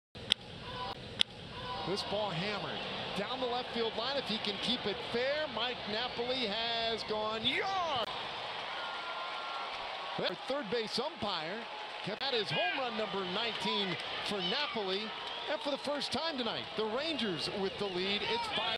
Mike Napoli crushes a go ahead two run home run
Mike Napoli pinch hits and connects on a long two-run home run to left field to give the Rangers a 5-3 lead in the top of the 7th inning